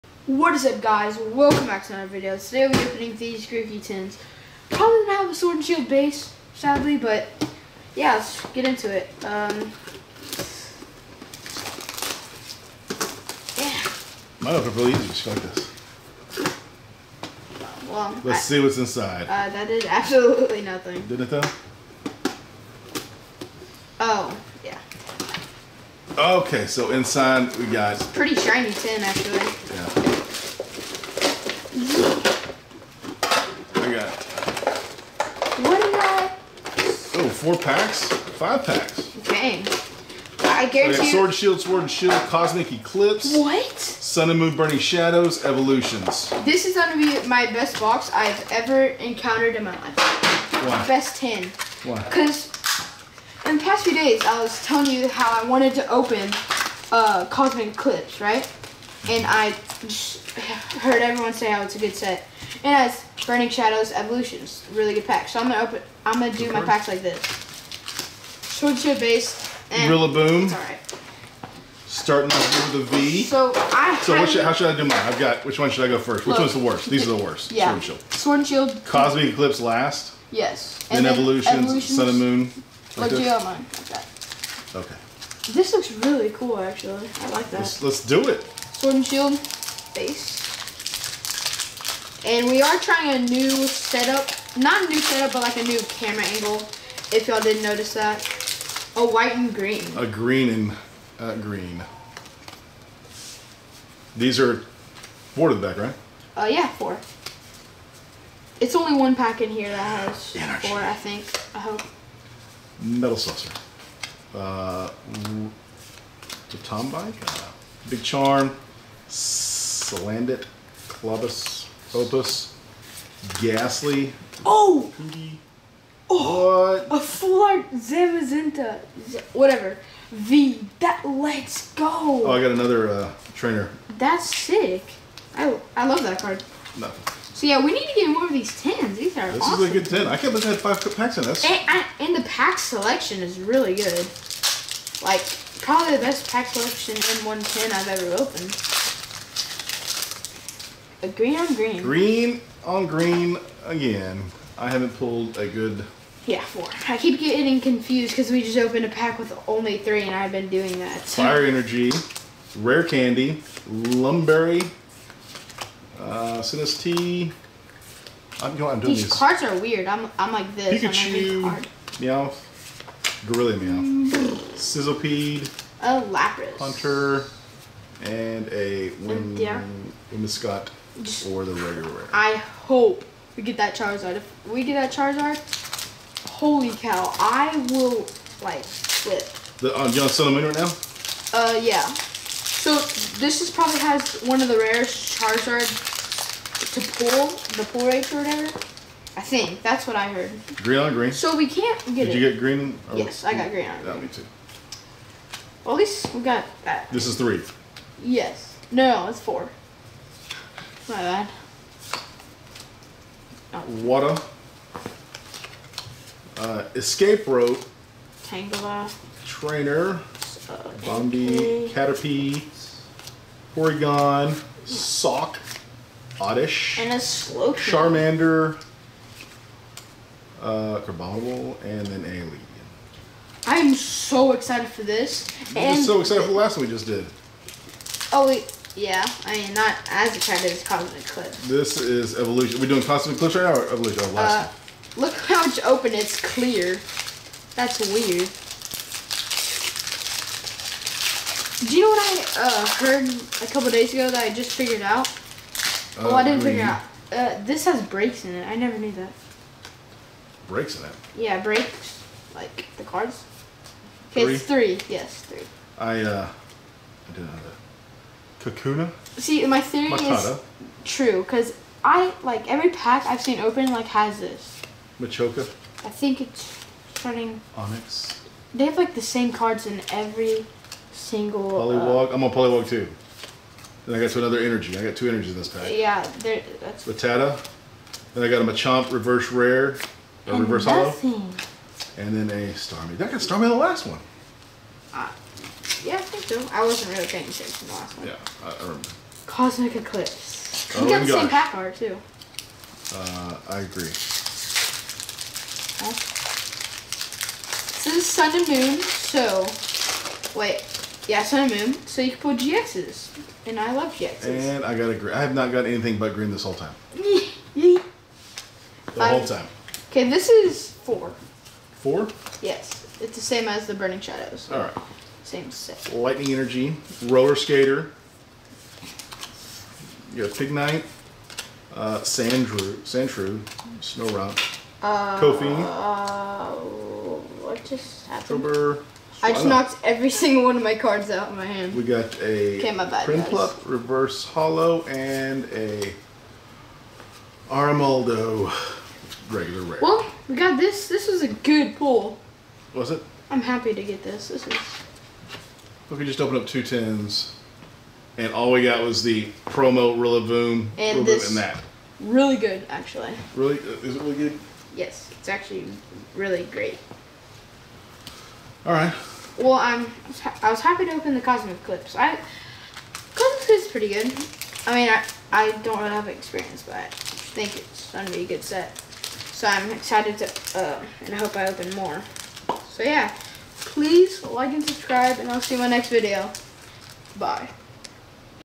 Opening 2 Pokémon Galar Tins
Today we are opening 2 Grokey / RillaboomV Tins from Galar